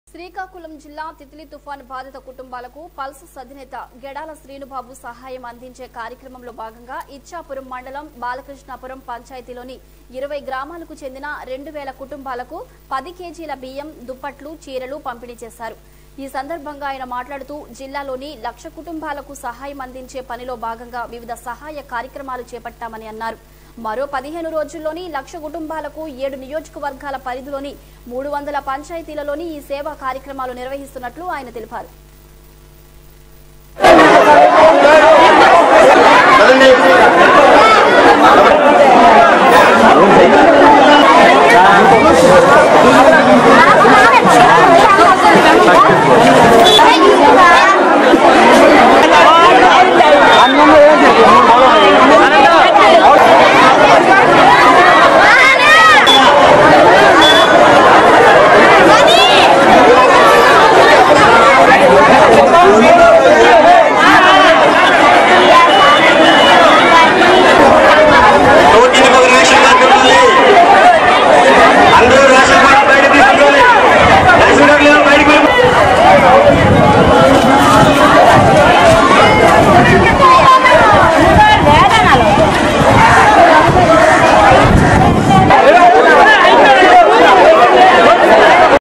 శ్రీకాకుళం జిల్లా టైటిలి తుఫాన్ బాధిత కుటుంబాలకు పల్సస్ అధినేత గెడలా శ్రీనుబాబు సహాయం అందించే
శ్రీకాకుళం జిల్లా టైటిలి తుఫాన్ బాధిత కుటుంబాలకు పల్సస్ అధినేత గెడలా శ్రీనుబాబు సహాయం అందించే కార్యక్రమంలో భాగంగా ఇచ్ఛాపురం మండలం బాలకృష్ణాపురం పంచాయతీలో 20 గ్రామాలకు చెందిన 2 వేళా కుటుంబాలకు 10 కే.జి.ల బియ్యం, దుప్పట్లు, చీరలు పంపిణి చేసారు. ఈ సందర్భంగా ఆయన మాట్లాడుతూ జిల్లాలో లక్ష కుటుంబాలకు సహాయం అందించే పనిలో భాగంగా వివిధ సహాయ కార్యక్రం చేపట్టామని అన్నారు. మరో 15 రోజుల్లో లక్ష కుటుంబాలకు 7 నియోజకవర్గాల పరిధిలో 300 పంచాయతీలలో ఈ సేవా కార్యక్రమాలు నిర్వహిస్తున్నట్లు తెలిపారు.